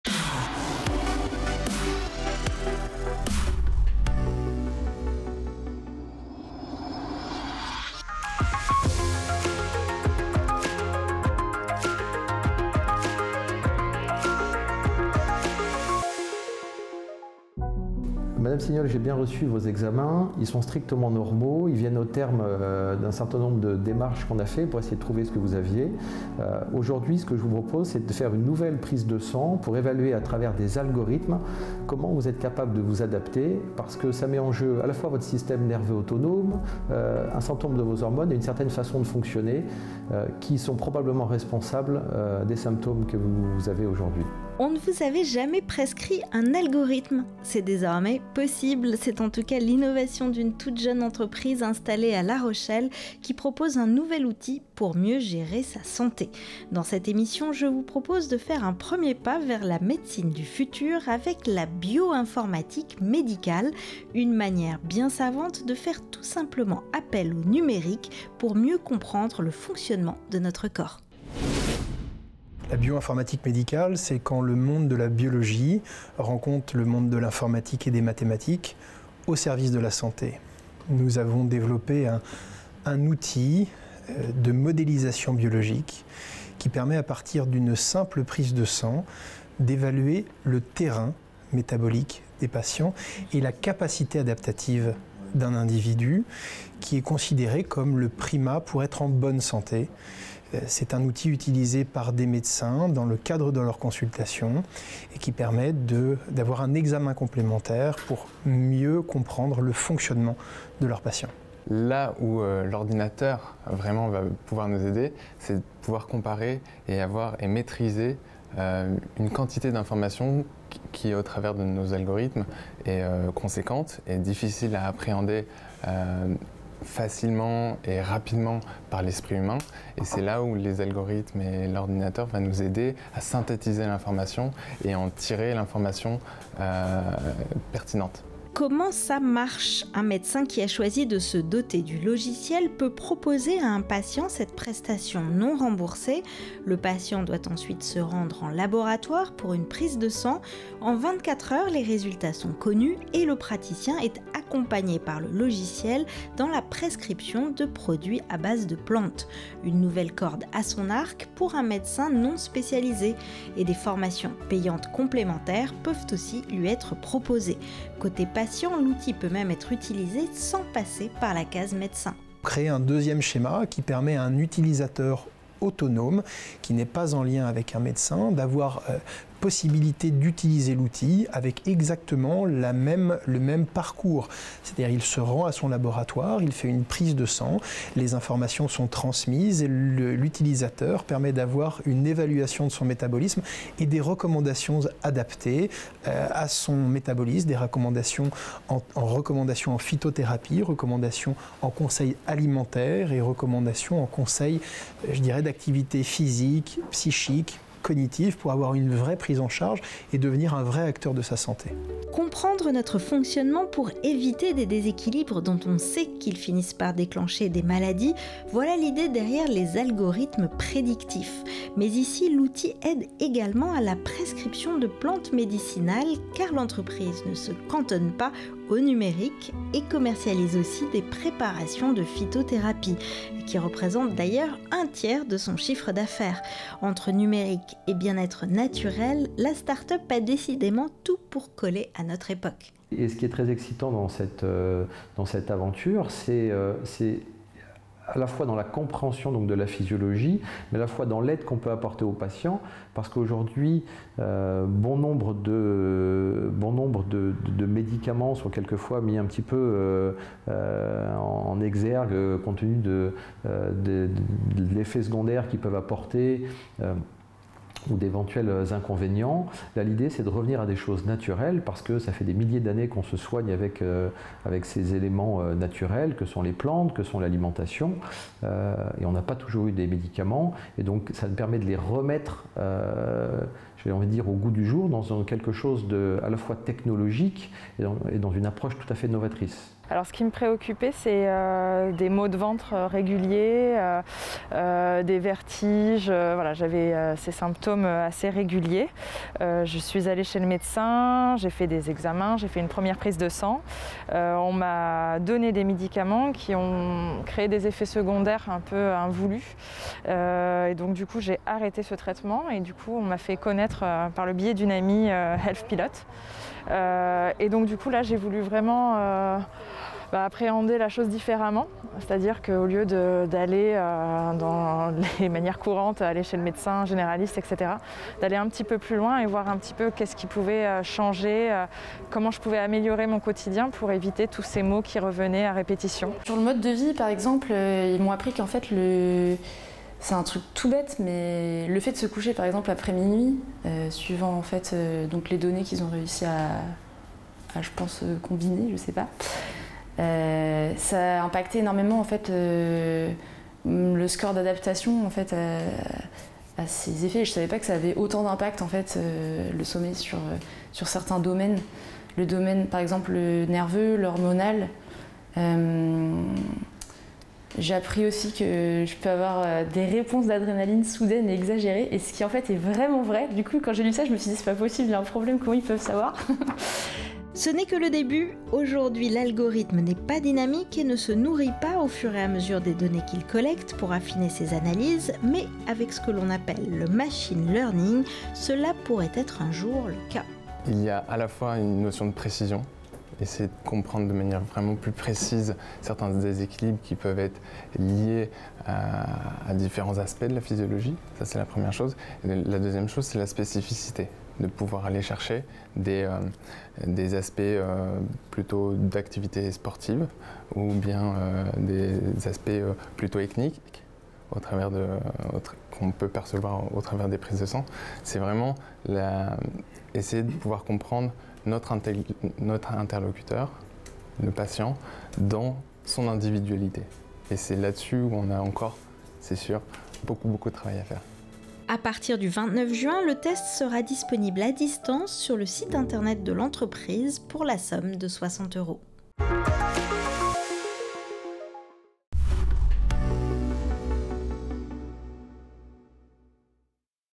Innovations - La bio-informatique médicale : l'avenir de la santé ?
Des algorithmes prédictifs pour prévenir les maladies et mieux comprendre la manière individuelle dont notre corps s'adapte à son environnement : cela fait un peu futuriste, mais c'est l'idée d'une start-up basée à La Rochelle qui vient d'ouvrir un site internet nouvelle génération inspiré par la télémédecine.
Le numérique sera-t-il incontournable dans la médecine de demain ?